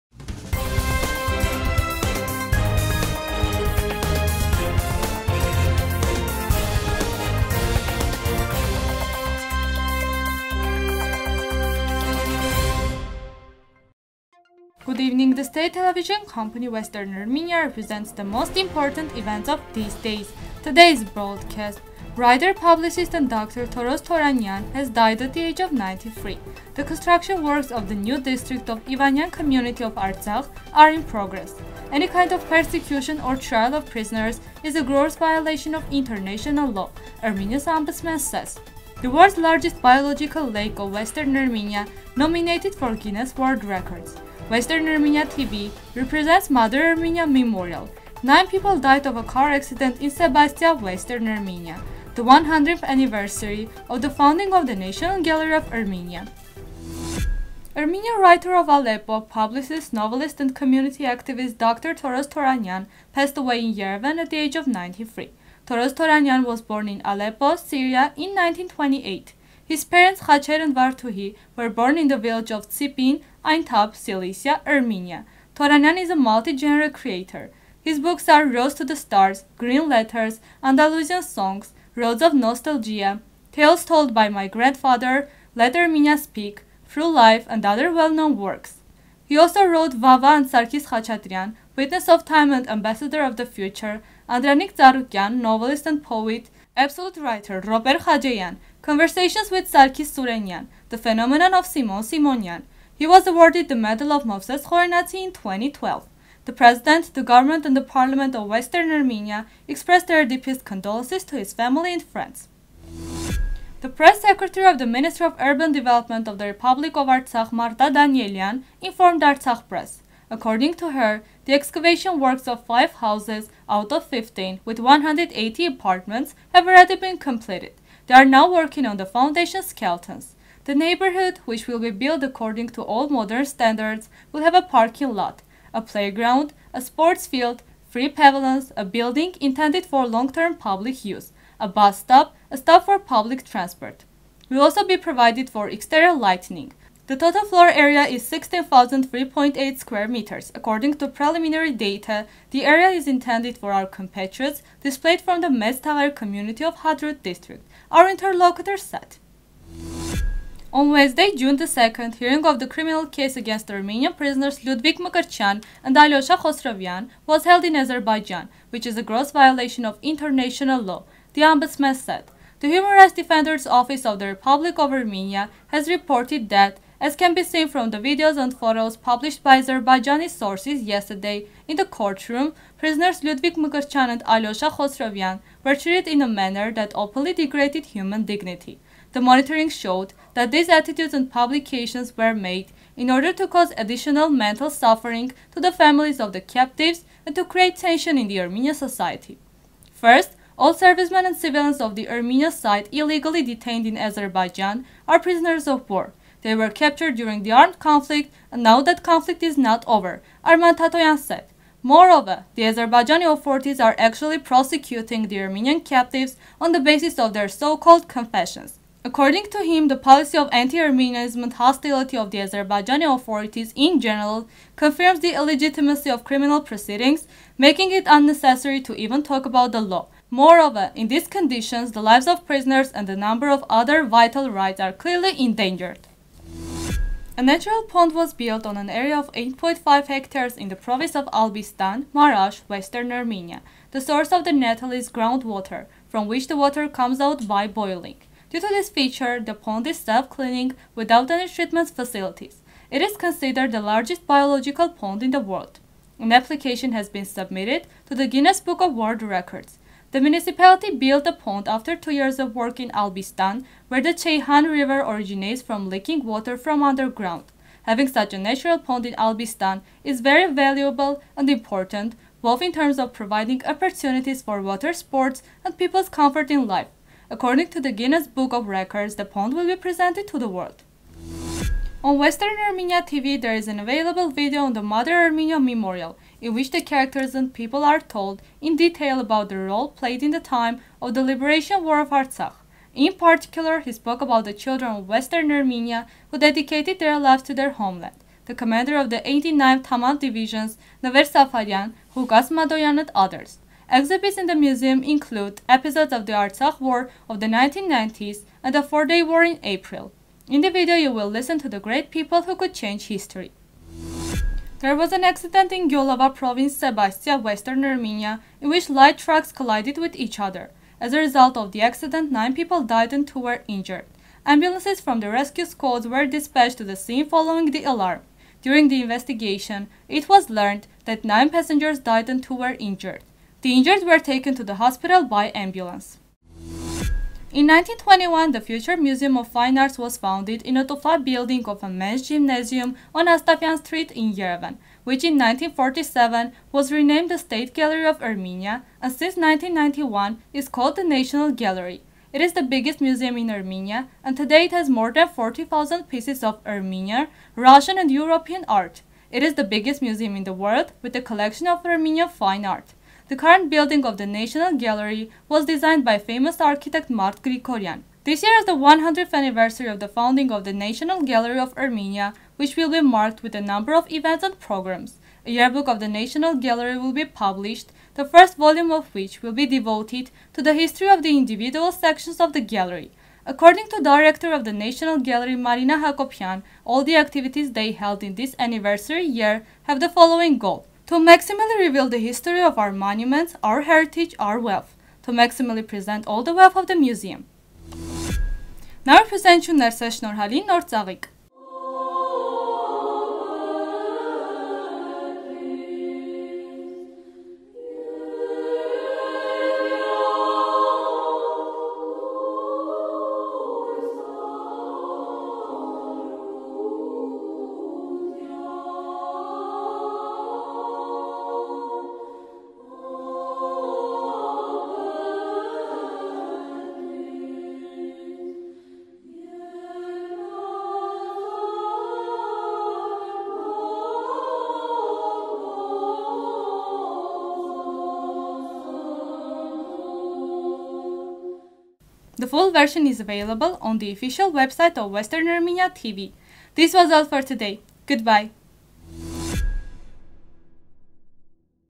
Toros Toranyan has died at the age of 93.News 04 06 2021
1.Writer, publicist and doctor Toros Toranyan has died at the age of 93
2.The construction works of the new district of Ivanyan community of Artsakh are in progress
3.Any kind of persecution or trial of prisoners is a gross violation of international law, Armenia’s Ombudsman says
4.The World's Largest Biological Lake of Western Armenia Nominated for Guinness World Records
5.Western Armenia TV represents ‘‘Mother Armenia’’ Memorial
6. 9 People died of a Car Accident in Sebastia, Western Armenia
7.The 100th anniversary of the founding of the National Gallery of Armenia